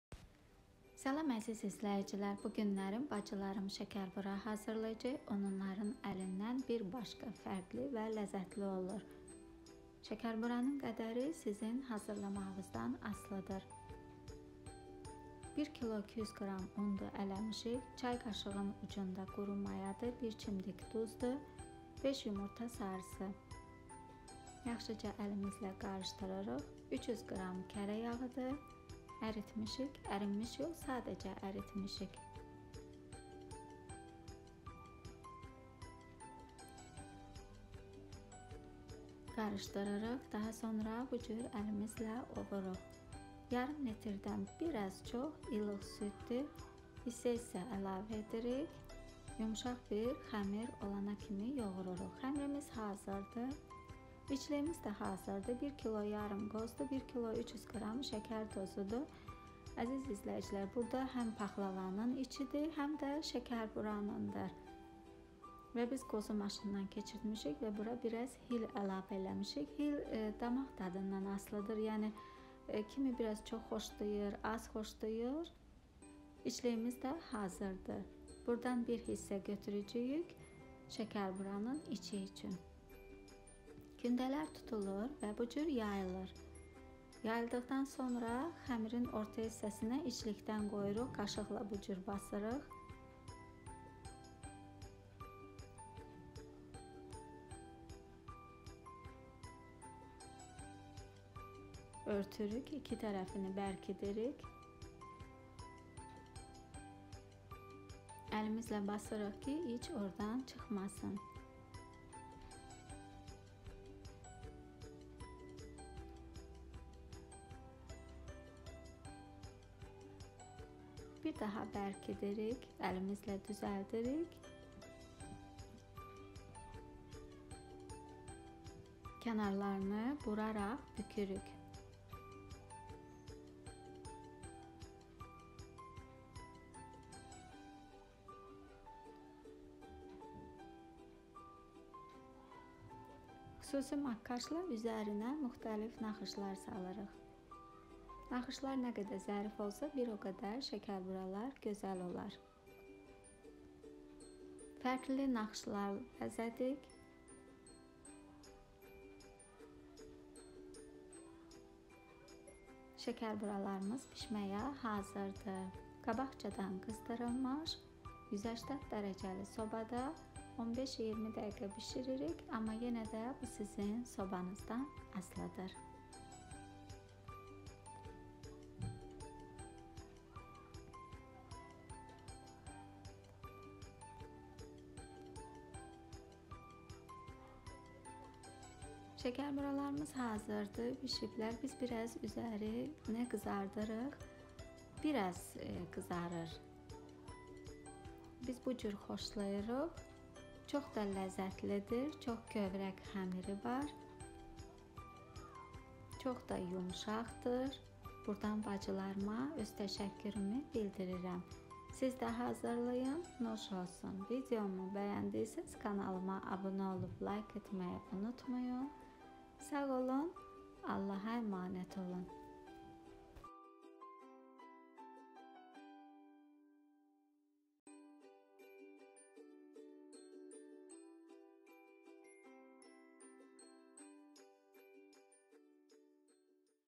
ŞEKERBURANIN HAZIRLANMASI ÇOX ZERİF VE YUMŞAQDIR
1.200q un
5yumurta sarısı
300q kere yağı
Cay qaşığının ucunda quru maya
Bir çimdik duz
Yarım litrden bir az çox ilıq süd
İçlik:
1.500q qoz
1.300q şeker tozu
Biraz hil
Amma bu hem şekerburanın içidir , hemde paxlavanın
180d sobada 15-20deq bişiririk Nuş olsun